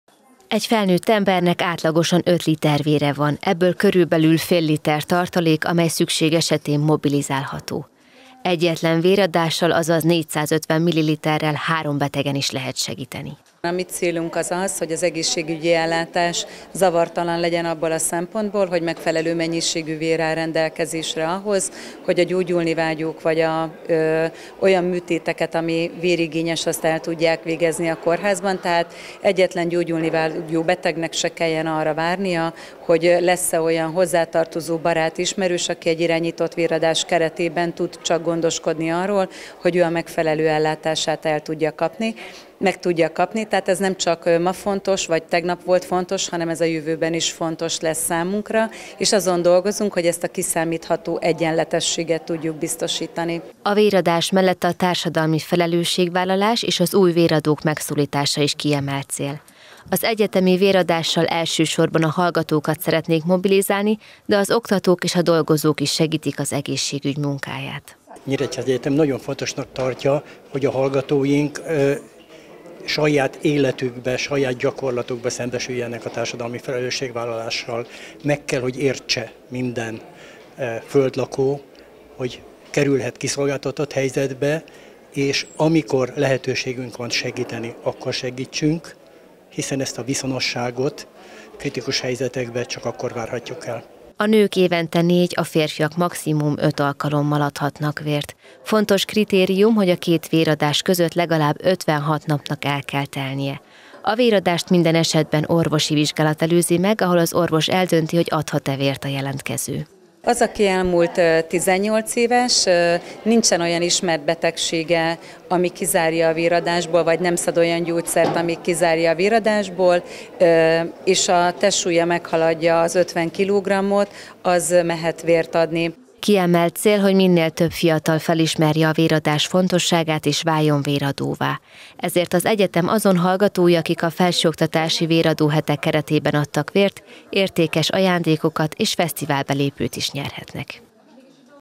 Véradás a Nyíregyházi Egyetemen
Két napon keresztül várták a véradókat a Nyíregyházi Egyetemen. A Magyar Vöröskereszt Szabolcs-Szatmár-Bereg Vármegyei Szervezete és az Országos Vérellátó Szolgálat által rendezett esemény egyik célja az volt, hogy minél több fiatalt bevonjanak a véradók közé.